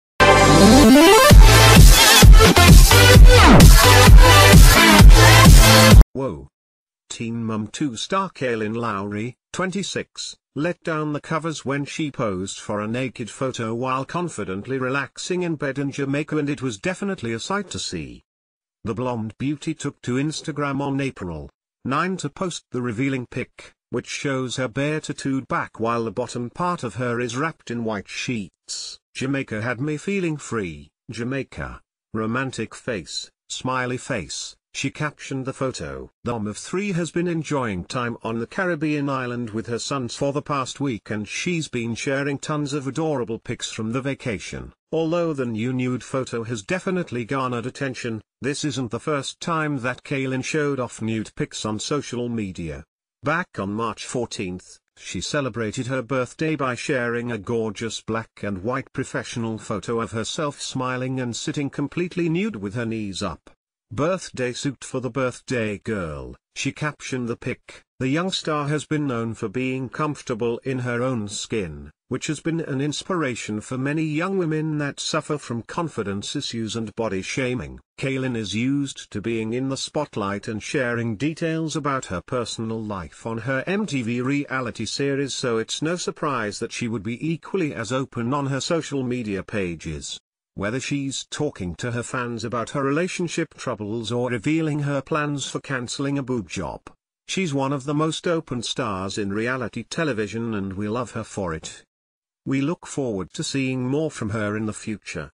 ‘Teen Mom’ Kailyn Lowry Gets Naked In Bed & Shares The Racy Pic With Her Fans
Woah! Teen Mom 2 star Kailyn Lowry, 26, let down the covers when she posed for a naked photo while confidently relaxing in bed in Jamaica and it was definitely a sight to see! The blonde beauty took t...